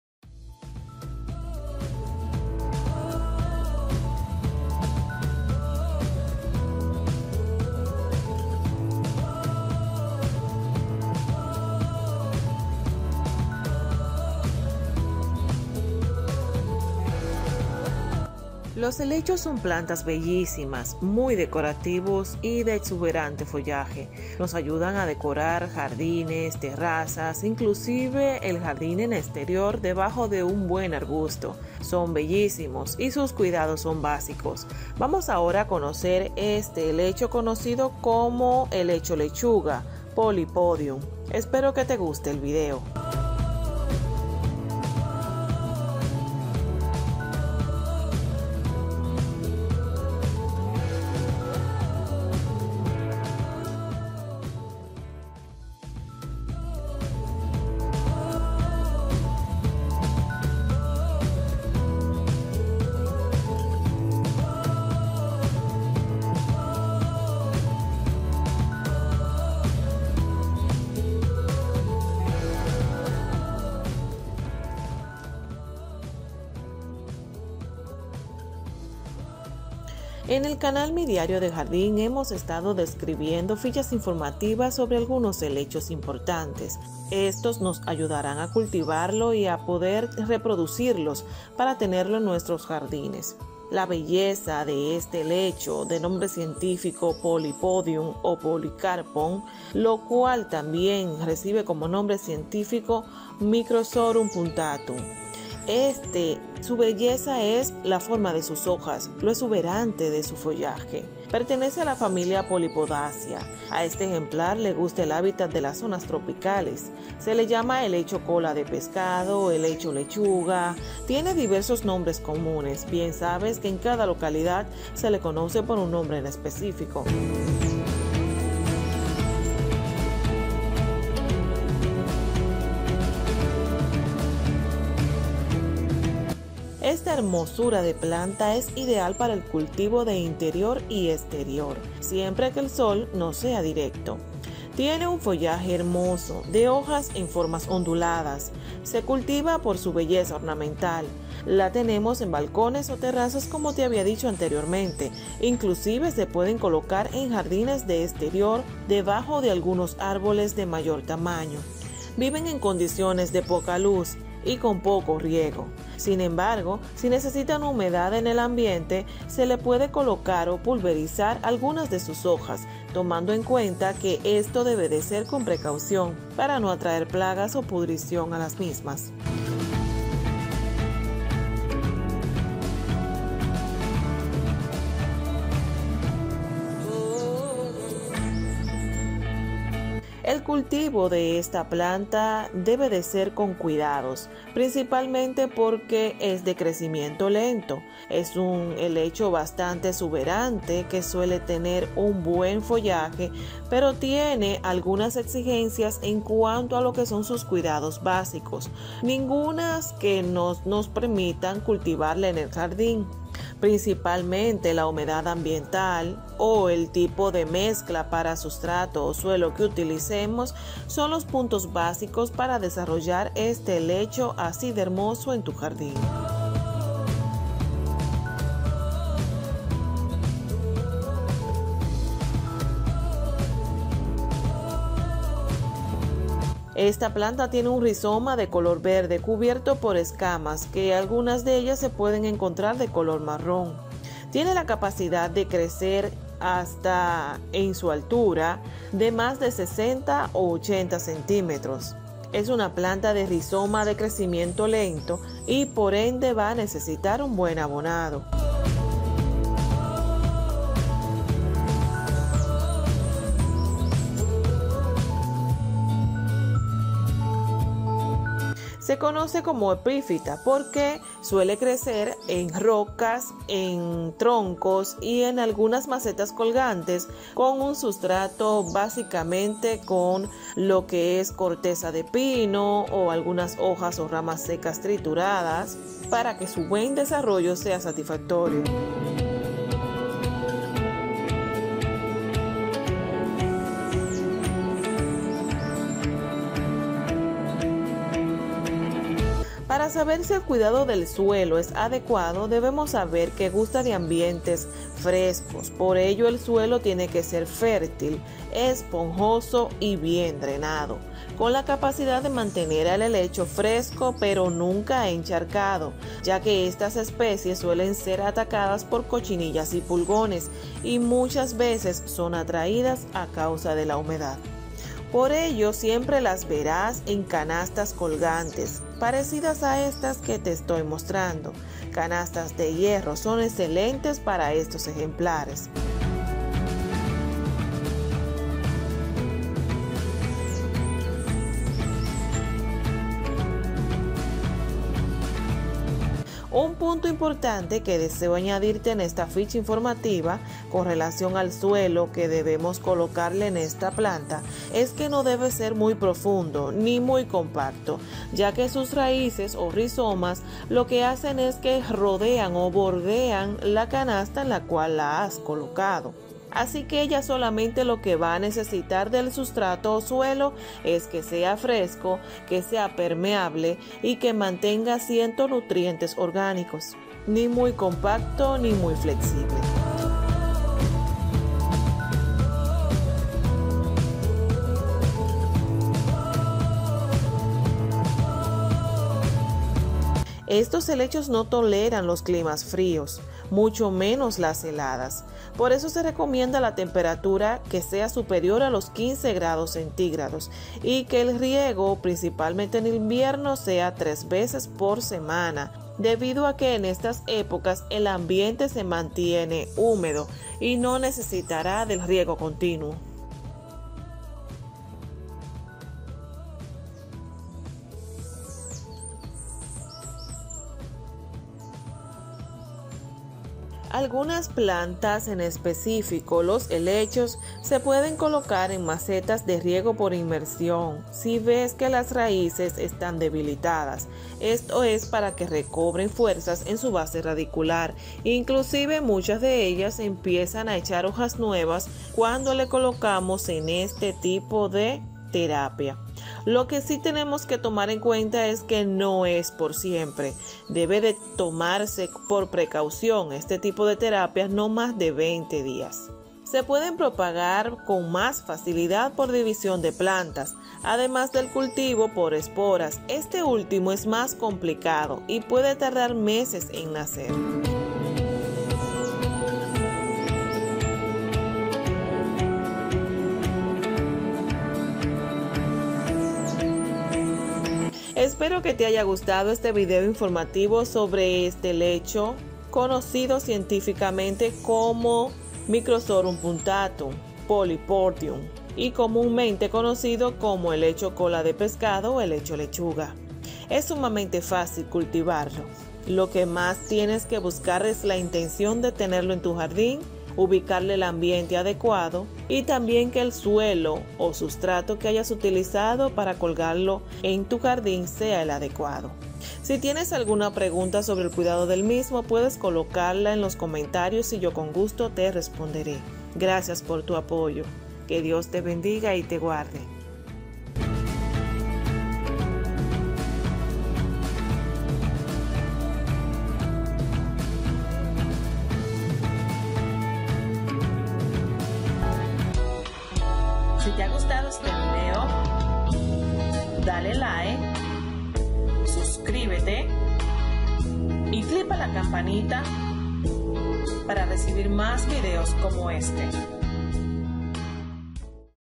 CUIDADOS BÁSICOS HELECHO LECHUGA, helecho Cristal, mi diario de jardin
CUIDADOS BÁSICOS HELECHO LECHUGA, helecho Cristal

Este helecho tiene la capacidad de crecer en altura  entre 60 y 80 CM. es una planta de rizoma de crecimiento lento y fácil que crece como una epífita que se adhiere mejor a las rocas o la madera. te muestro sus cuidados basicos.

🌺tiene un follaje hermoso de hojas en forma ondulada, se cultiva por su belleza ornamental, las tenemos en balcones o terrazas y en el exterior en jardines pero debajo de árboles de mayor tamaño, Viven en condiciones de poca luz y con poco riego, sin embargo si necesitan humedad en el ambiente.